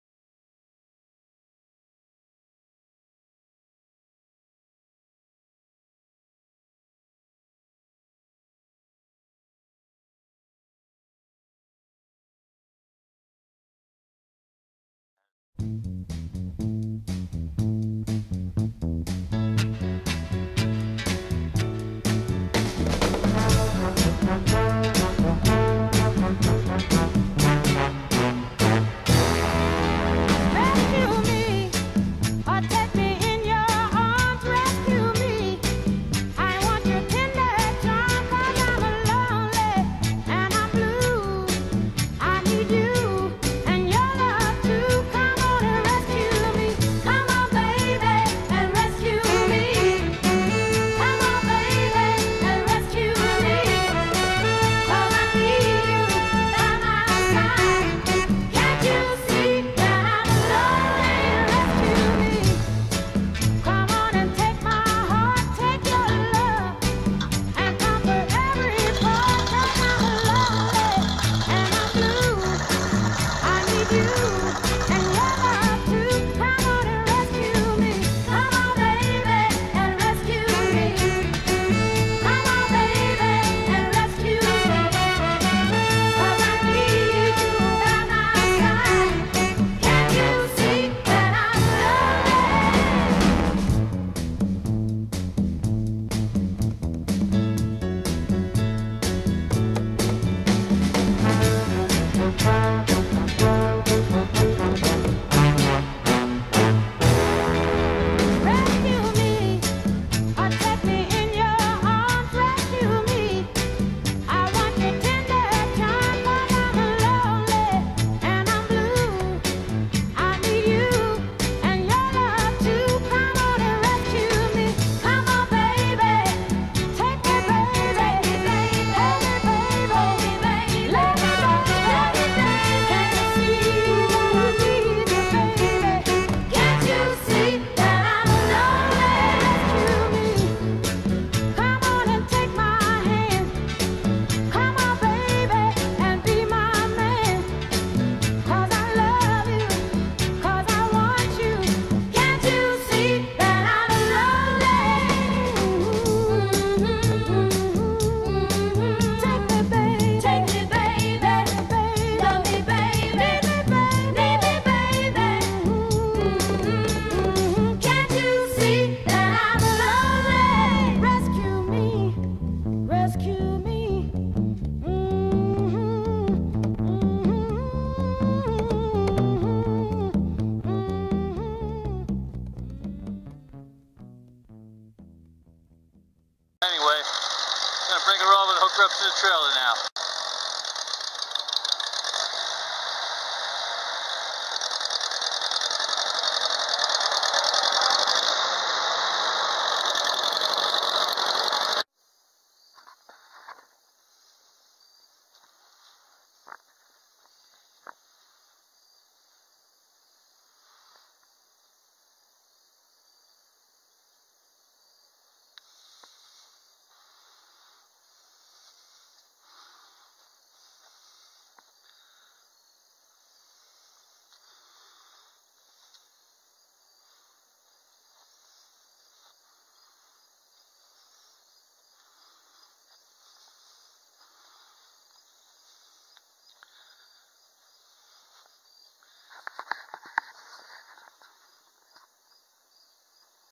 HAPPY TRACTOR RESCUE STORY.....#1
***PLEASE READ . SO, i DECIDED TO MAKE A COLLECTION OF MY TRACTOR RESCUE STORIES TO PASS ON TO MY GRANDKIDS, SO THEY WILL KNOW , AS PAUL HARVY WOULD SAY..."THE REST OF THE STORY".. OF THE OLD TRACTORS THEY ALL LOVED TO PLAY ON WHEN THEY WERE KIDS. WHO KNOWS, MAYBE ONE OF THEM MIGHT CATCH THE "TRACTOR FEVER" LIKE THEIR GRANDPA DID AND RESCUE ONE OF THEIR OWN... THANKS FOR WATCHING, I HOPE YOU ENJOY THE STORY. HERE IS A SAMPLE OF HER PROVING HOW GOOD SHE REALLY IS.